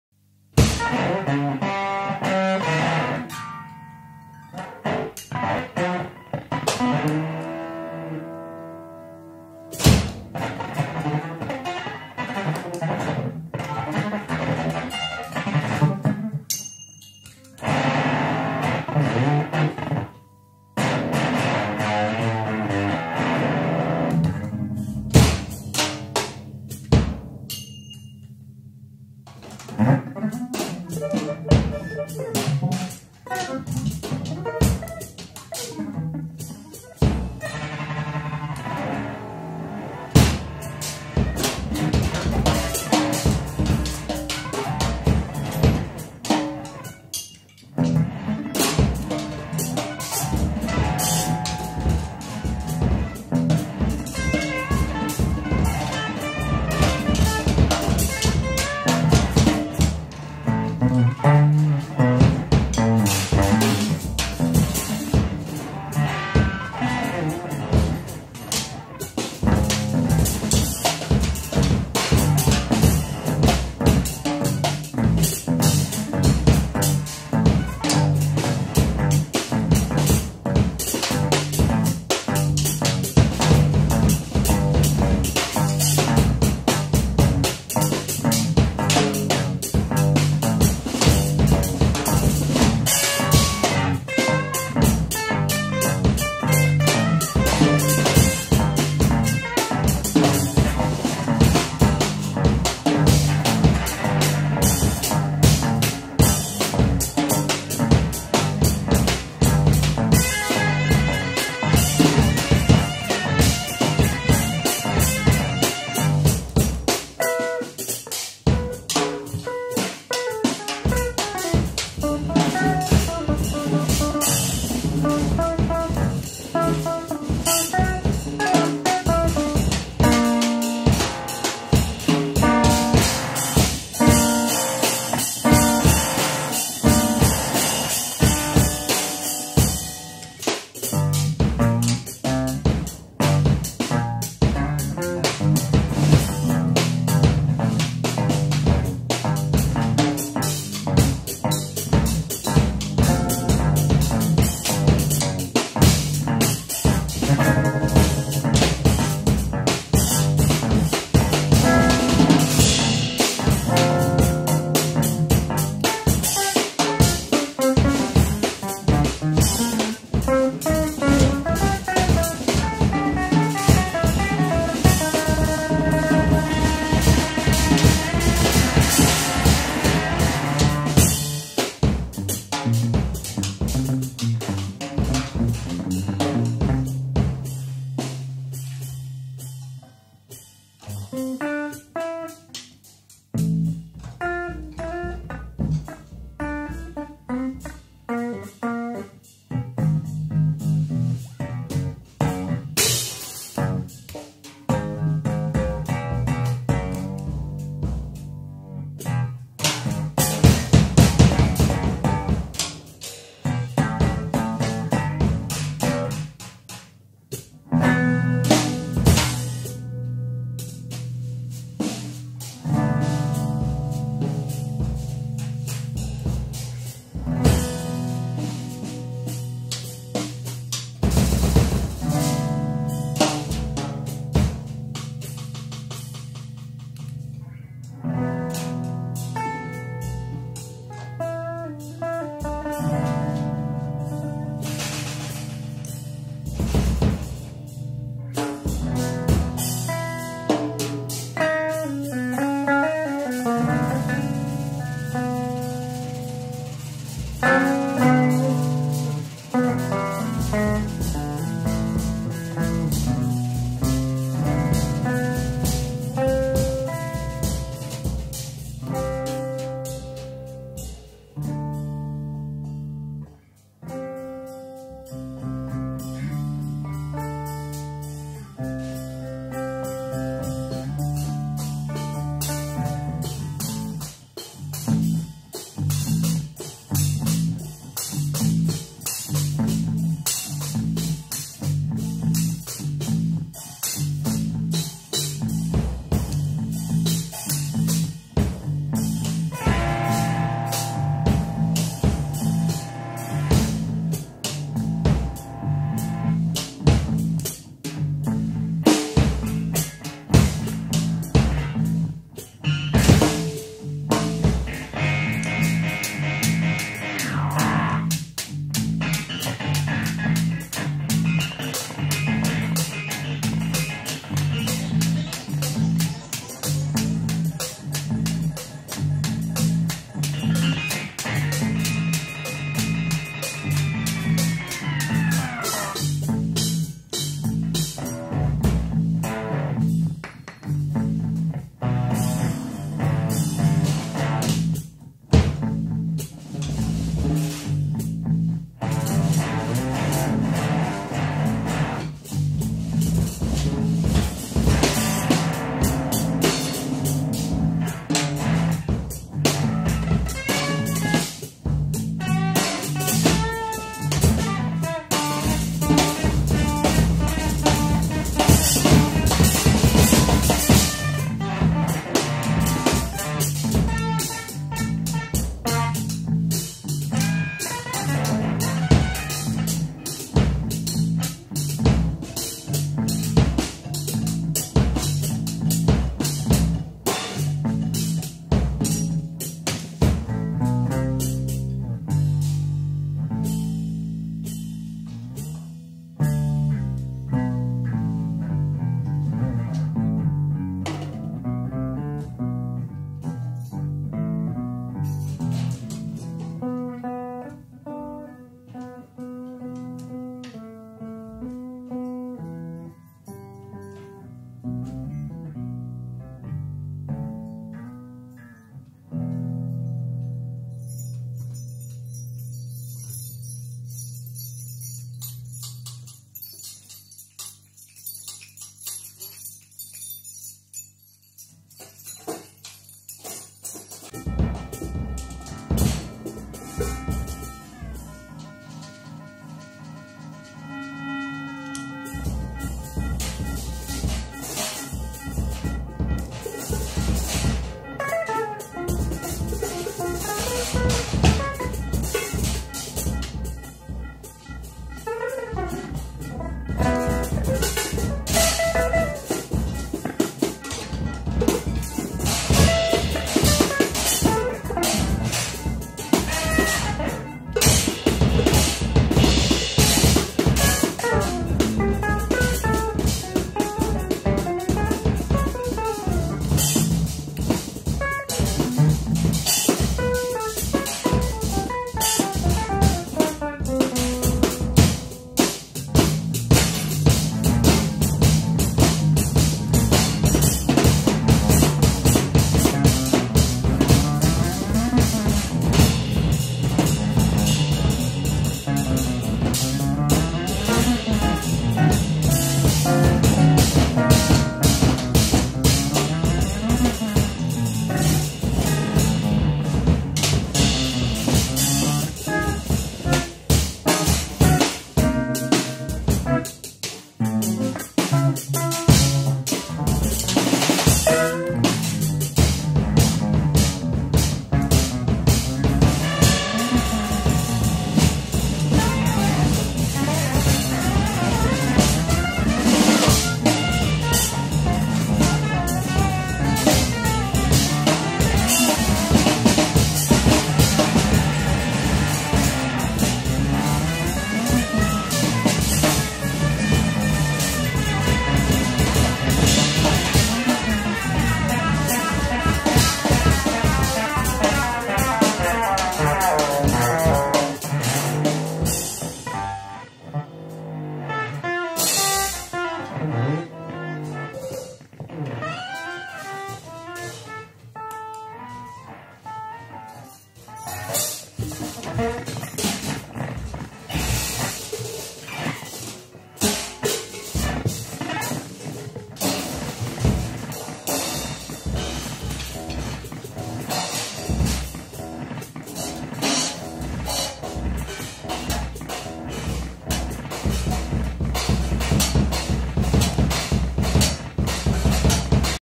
CHIPMUNK Live at WIM
Sergio Beresovsky on Guitar and Daniel Disler on Drums